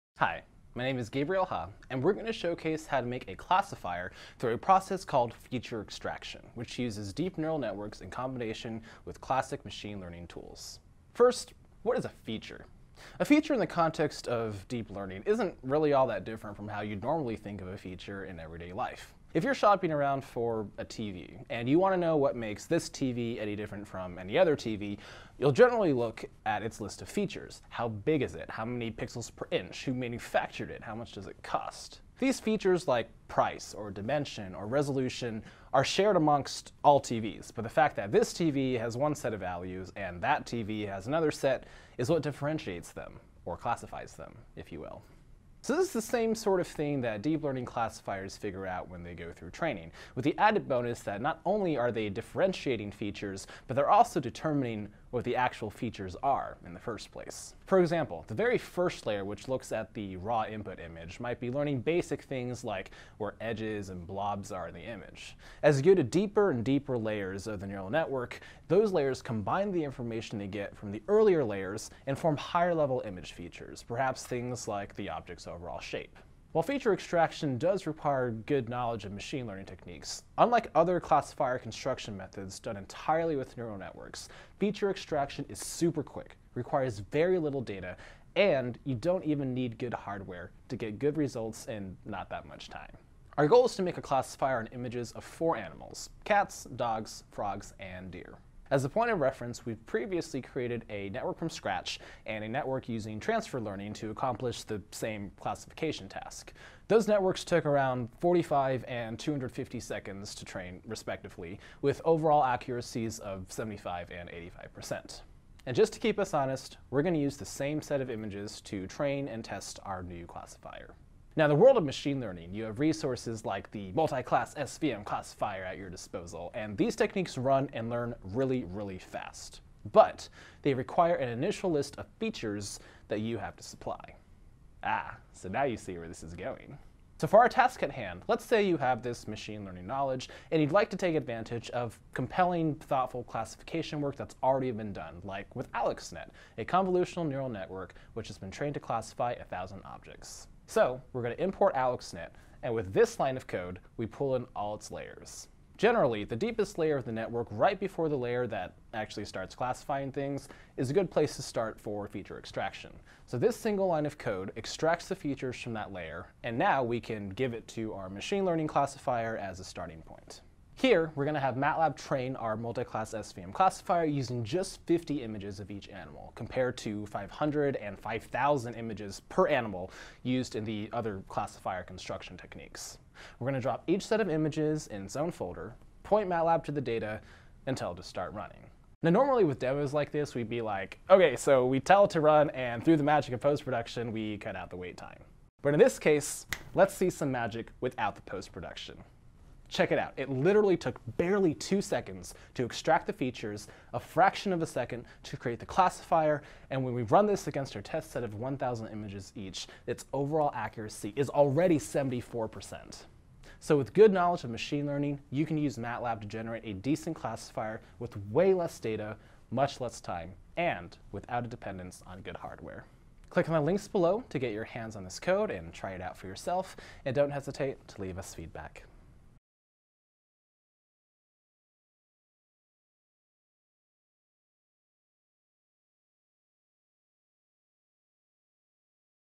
Deep Learning with MATLAB: Using Feature Extraction with Neural Networks in MATLAB
This demo uses MATLAB® to train a SVM classifier with features extracted, using a pretrained CNN for classifying images of four different animal types: cat, dog, deer, and frog. Images are used from the CIFAR-10 dataset